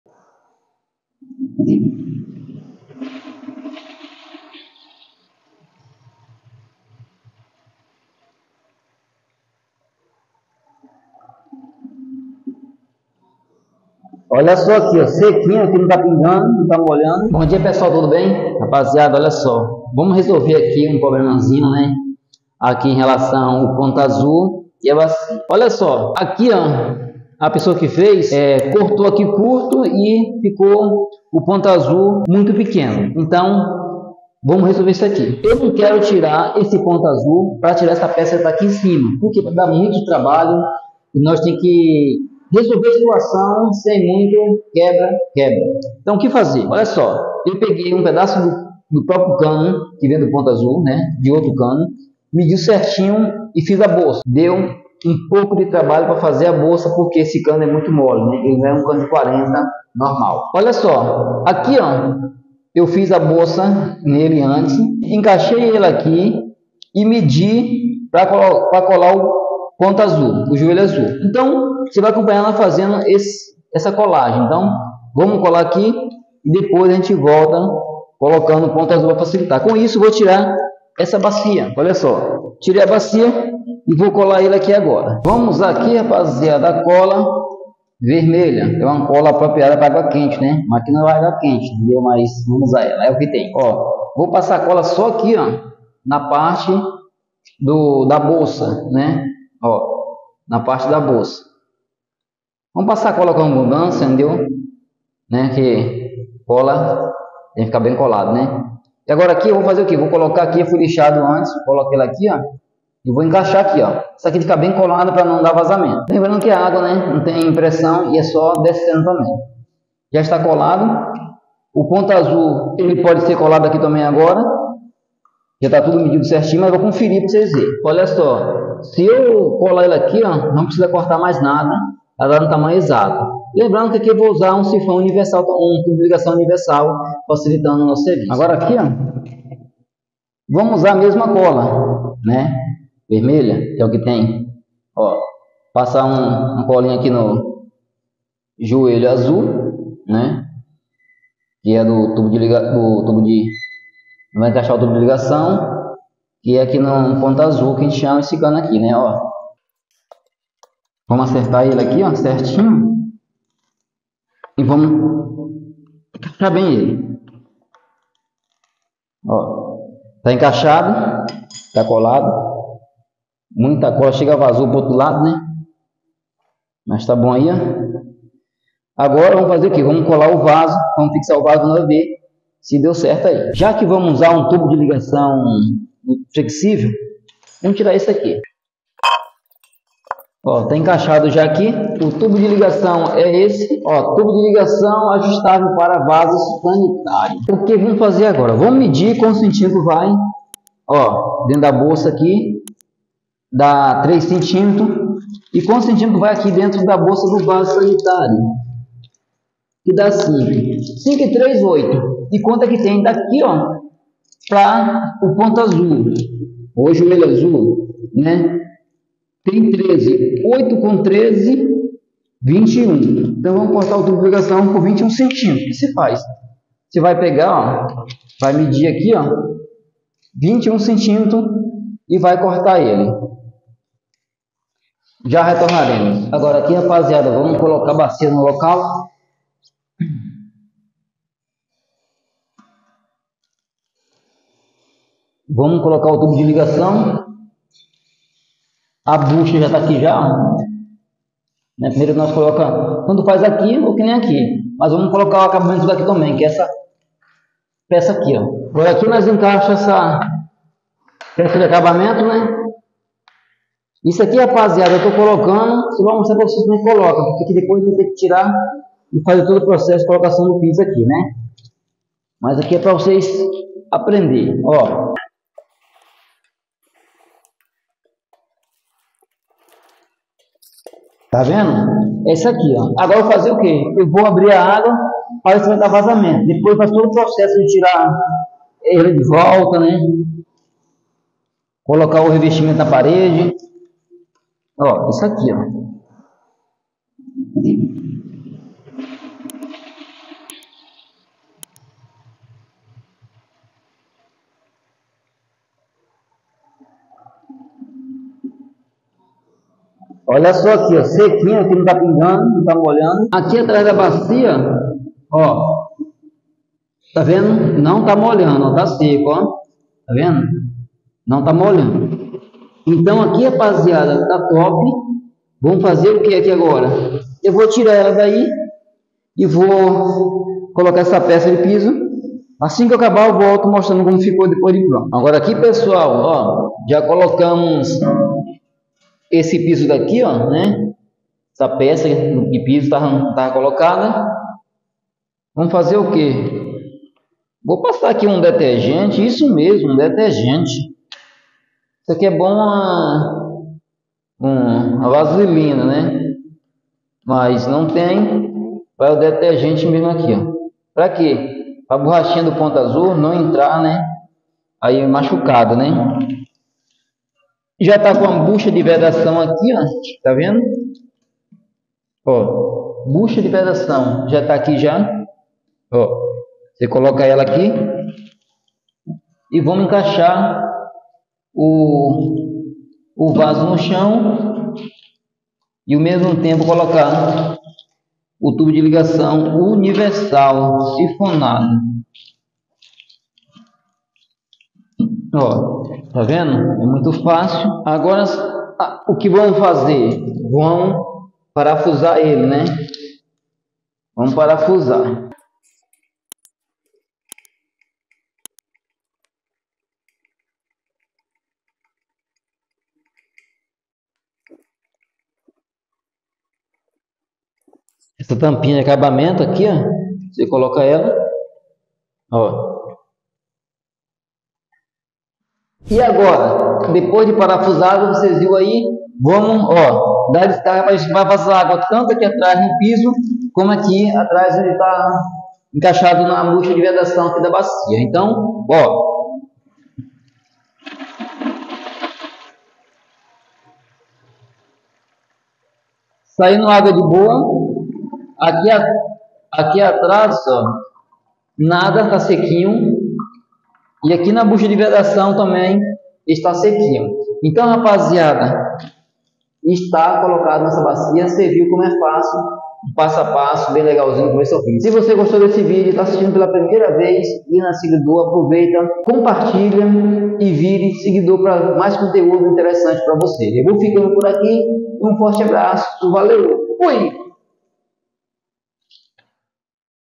Bacia sanitaria colocada errada! Veja como Resolver
Bacia sanitaria colocada errada! Veja o que fazer para resolver esse problema.